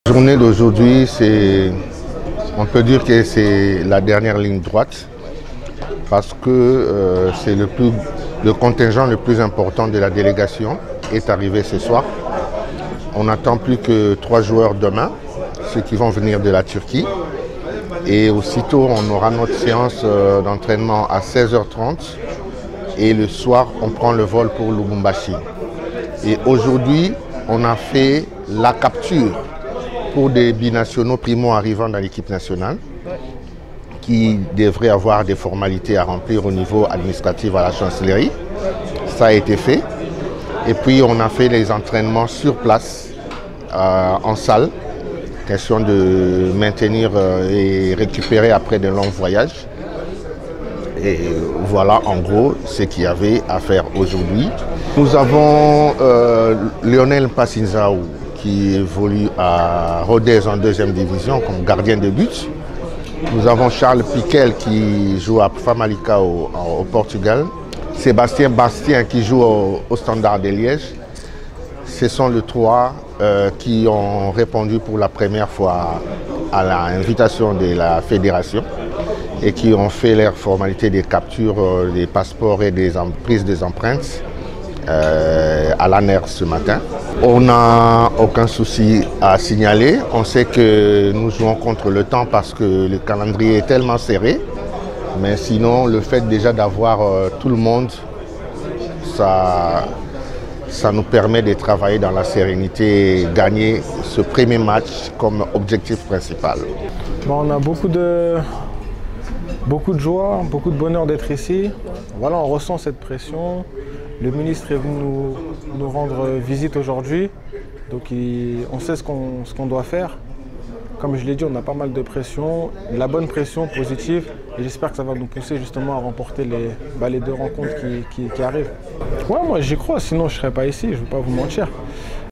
LES LEOPARDS QUI SONT PRÉSENTS À KINSHASA POUR LES ÉLIMINATOIRES COUPE DU MONDE QATAR 2022.
Infos_Leopards Place à la sélection

Le Manager des leopards Dodo Landu fait les points sur l'effectif présent et sur les 3 nouveaux joueurs qui ont rejoints la tanière des leopards pour la première fois et leur démarche administrative qui s'est bien passée.

Cedric Bakambu a également été interviewé à la fin de la vidéo.